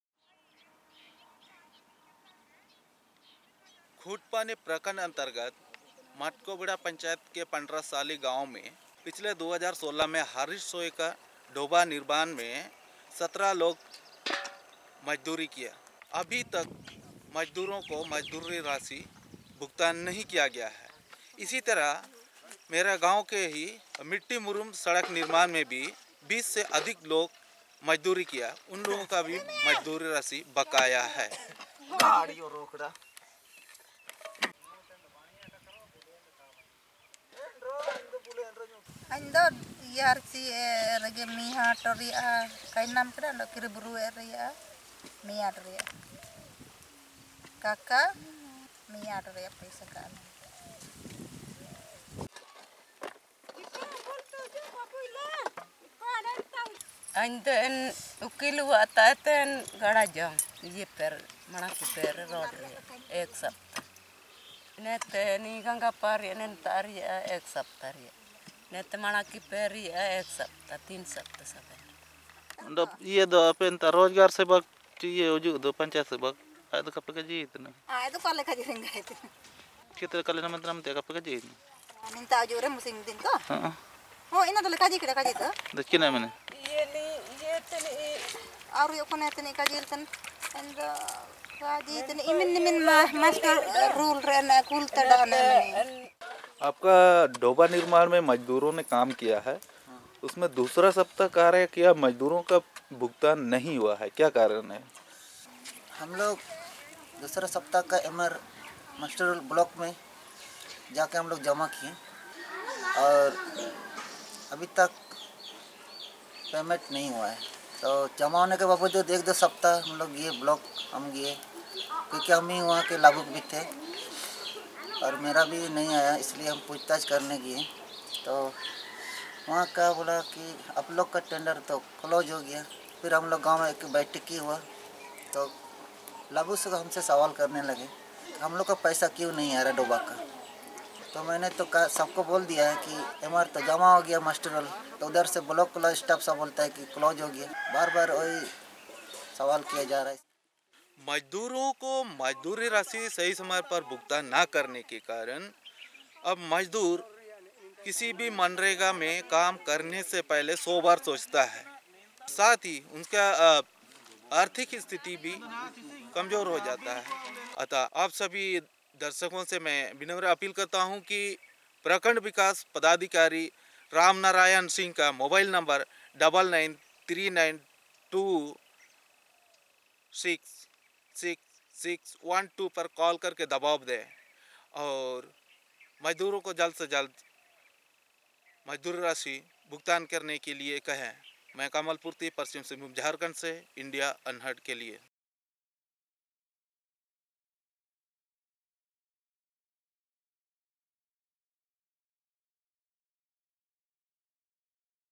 No wages for MNREGA workers
Kamal Kishor Purty reports from West Singhbhum, Jharkhand for Video Volunteers.